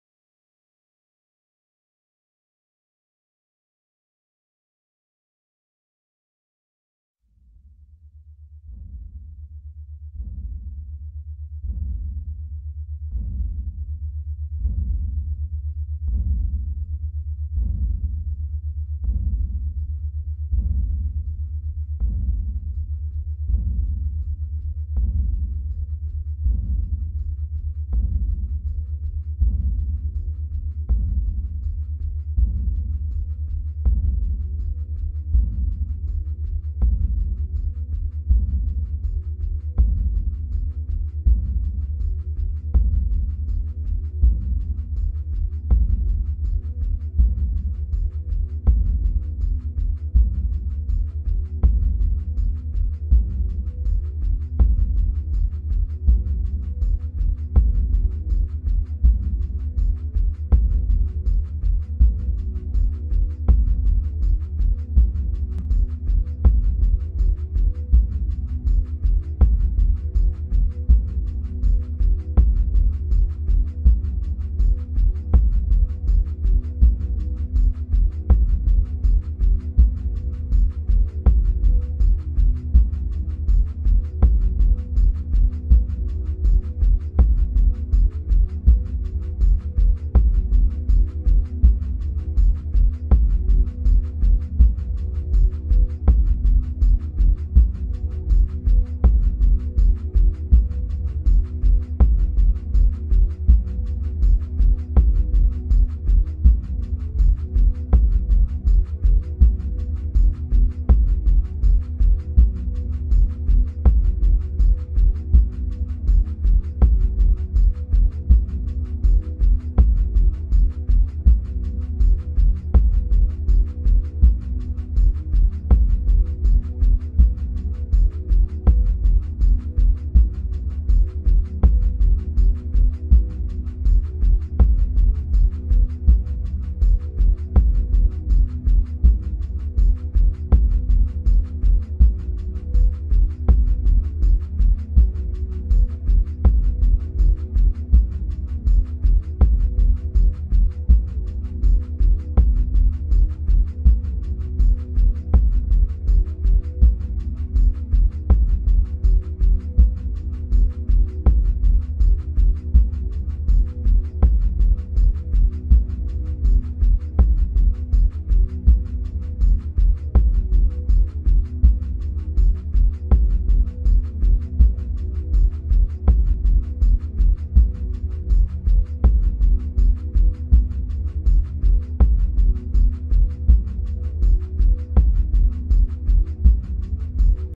Amazing New Archaeological Discovery In Australian Cave
The unusual sizes and shapes of these motifs raise questions about the types of material used for the stencil templates.
What stands out is a strange human-like figure, a huge face next to the human-like figure and an alien face at the bottom left of the image.